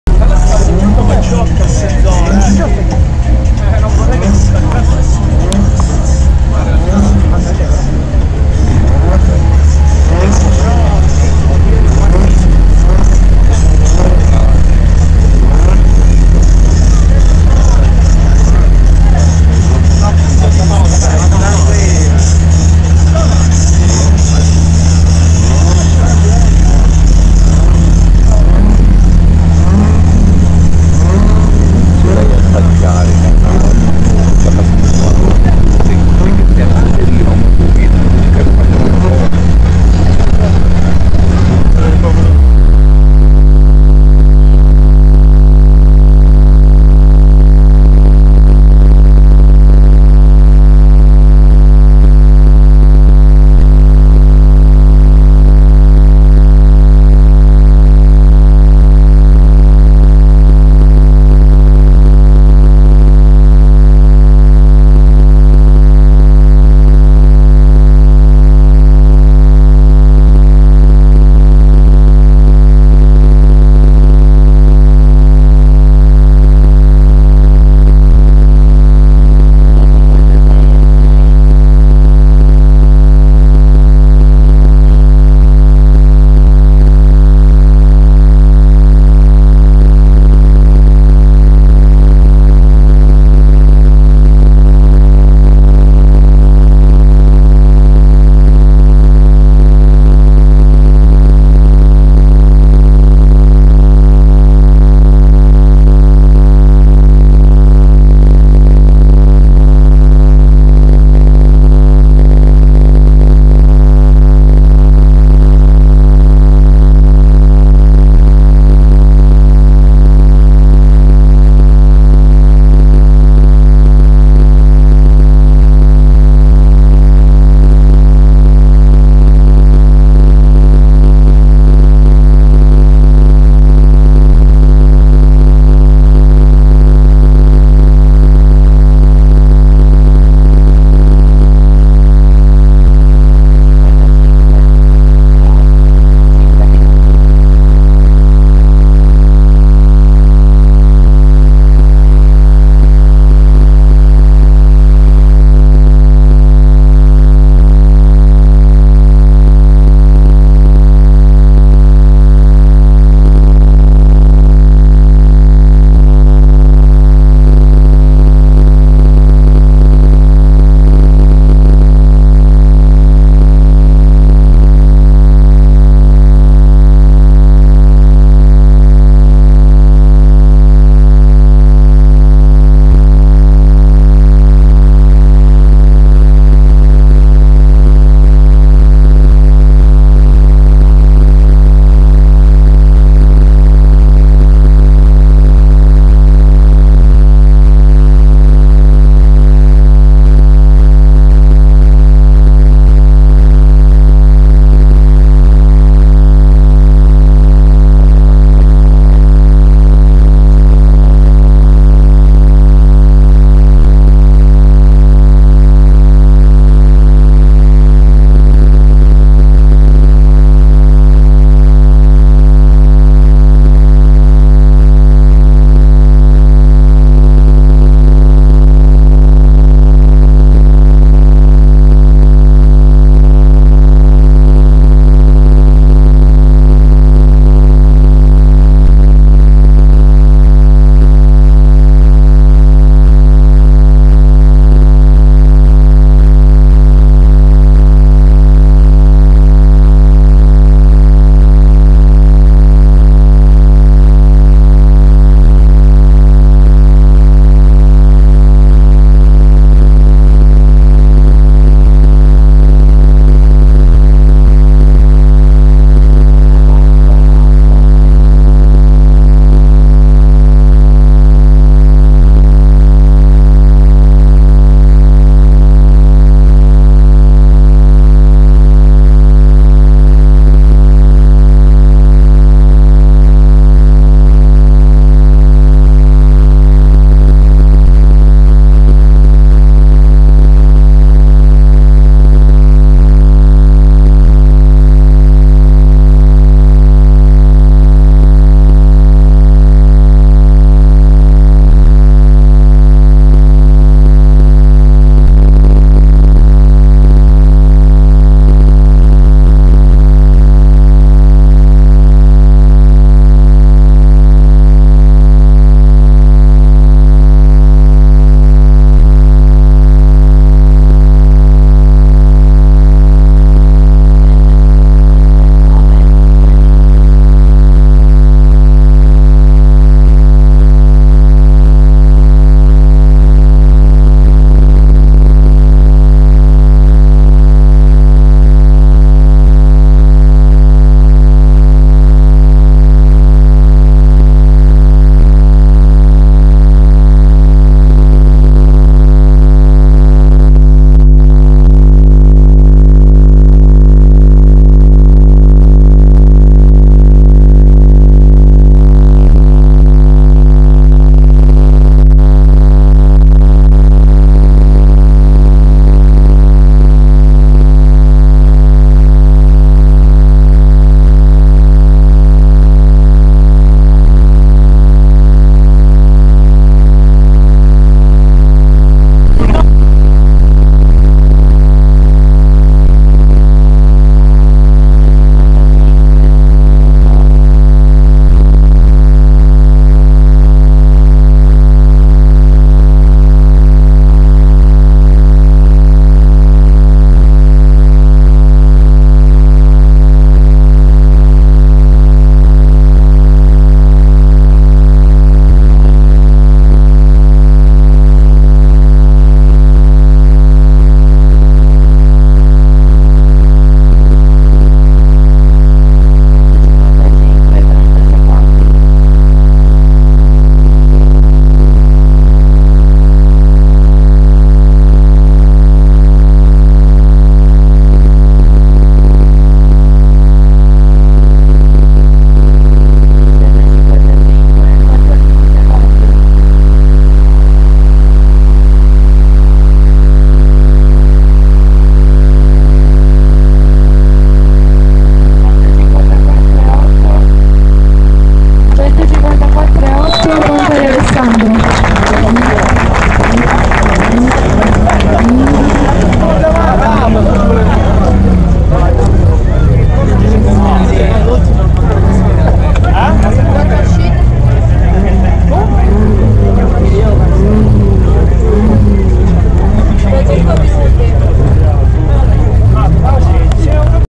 Pontari Hi Fi:LoudMachine 640. 154.8@50 hertz!! kicker l7+Steg K1.5000
risultato ottenuto con 8 kicker da 15" e 4 steg k1.5000D @154.8DB